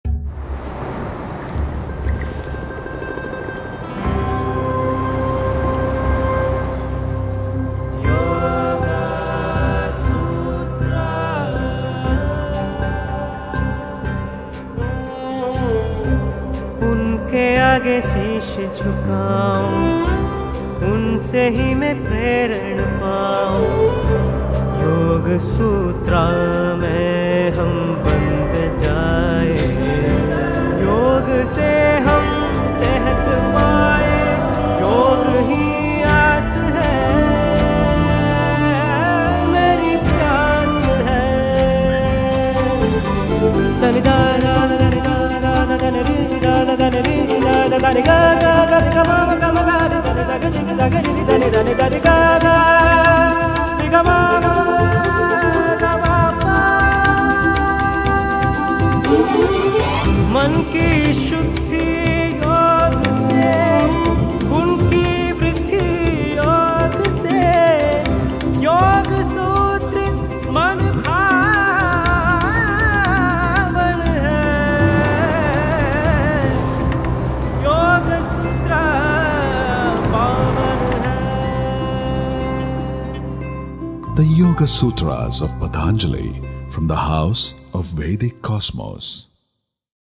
The Signature Edition of Sage Patanjali's Yoga Sutras (Official Promo Video) | Vedic Cosmos
Vedic Cosmos launches a Signature Edition of The Yoga Sutras of Patanjali. Composed thousands of years ago, this work is the foundation of India’s greatest legacy: the practice of Yoga.